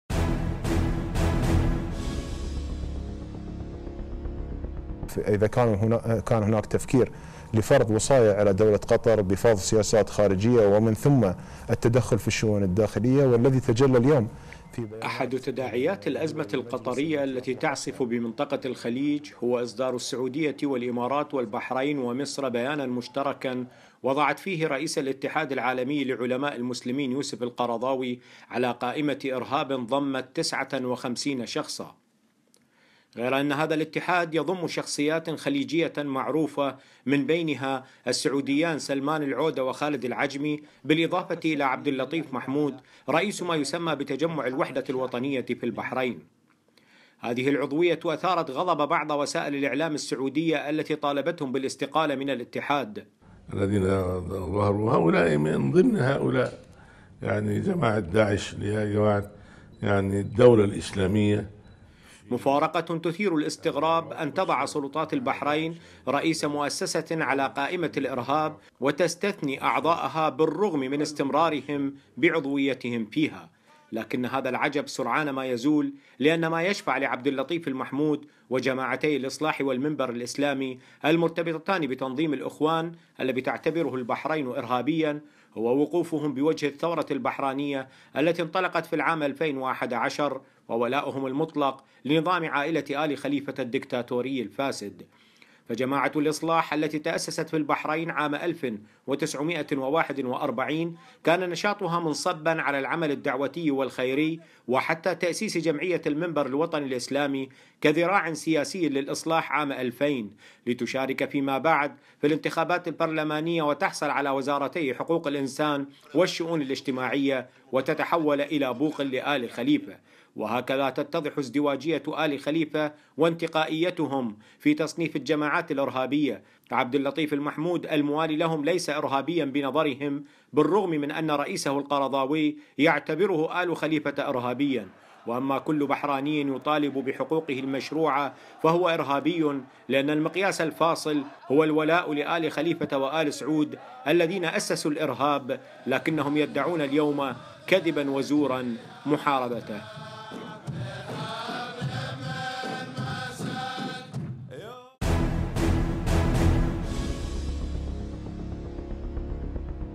تقرير متلفز: لماذا لا يضع آل خليفة عبداللطيف المحمود على قائمة الإرهاب؟‎
أحد تداعيات الأزمة القطرية التي تعصف بمنطقة الخليج هو إصدار السعودية والإمارات والبحرين ومصر بيانا مشتركا وضعت فيه رئيس الاتحاد العالمي لعلماء المسلمين يوسف القرضاوي على قائمة إرهاب ضمت 59 شخصا.

غير ان هذا الإتحاد يضم شخصيات خليجية معروفة من بينهم السعوديان سلمان العودة وخالد العجمي بالإضافة الى عبداللطيف محمود رئيس ما يسمى بتجمع الوحدة الوطنية في البحرين.
هذه العضوية أثارت غضب بعض وسائل الإعلام السعودية التي  طالبتهم بالإستقالة من الإتحاد. مفارقة تثير الإستغراب أن تضع سلطات البحرين رئيس مؤسسة على قائمة الإرهاب وتستثني أعضاءها بالرغم من استمرارهم بعضويتهم فيها.

لكن هذا العجب سرعان ما يزول لإن ما يشفع لعبداللطيف المحمود وجماعتي الإصلاح والمنبر الإسلامي  المرتبطتان بتنظيم الأخوان الذي تعتبره البحرين ارهابيا, هو وقوفهم بوجه الثورة البحرانية التي انطلقت في العام 2011 وولاؤهم المطلق لنظام عائلة آل خليفة الدكتاتوري الفاسد.

فجماعة الإصلاح التي تأسست في البحرين عام 1941 كان نشاطها منصبا على العمل الدعوتي والخيري وحتى تأسيس جمعية المنبر الوطني الإسلامي كذراع سياسي للإصلاح  عام ٢٠٠٠ لتشارك فيما بعد في الإنتخابات البرلمانية وتحصل على وزارتي حقوق الإنسان والشؤون اللإجتماعية وتتحول الى بوق لآل خليفه.
وهكذا تتضح إزدواجية آل خليفة وانتقائيتهم في تصنيف الجماعات الإرهابية فعبداللطيف المحمود الموالي لهم ليس ارهابيا بنظرهم بالرغم من ان رئيسه القرضاوي يعتبره آل خليفة إرهابيا واما كل بحراني يطالب بحقوقه المشروعة فهو ارهابي لأن المقياس الفاصل هو الولاء لآل خليفة وآل سعود الذين أسسوا الإرهاب لكنهم يدّعون  اليوم كذبا وزورا محاربته.